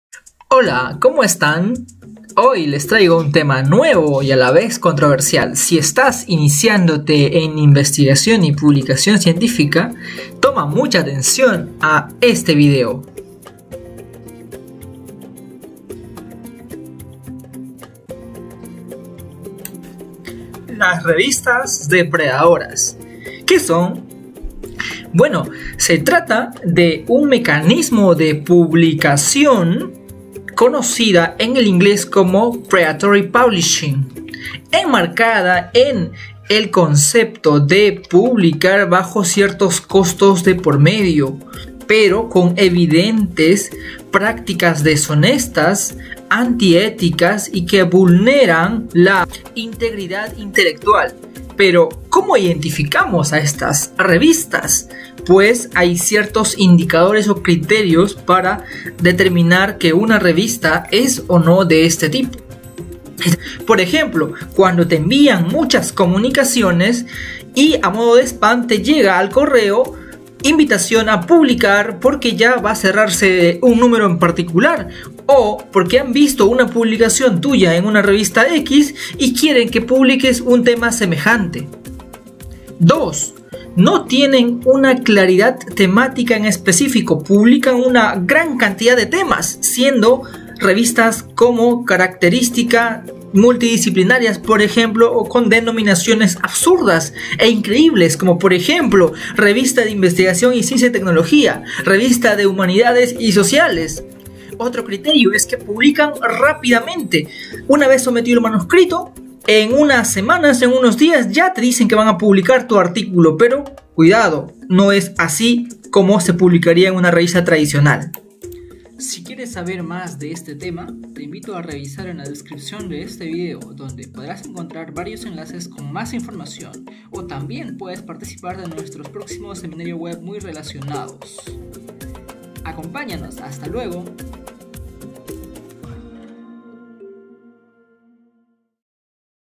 Comentario: Revistas depredadoras = Predatory Journals, ¿Qué son y cómo identificarlas?
Comentarios | Presentación sobre qué son y cómo identificar a las revistas depredadoras dirigido a personas que se estén iniciando en investigación y publicación científica.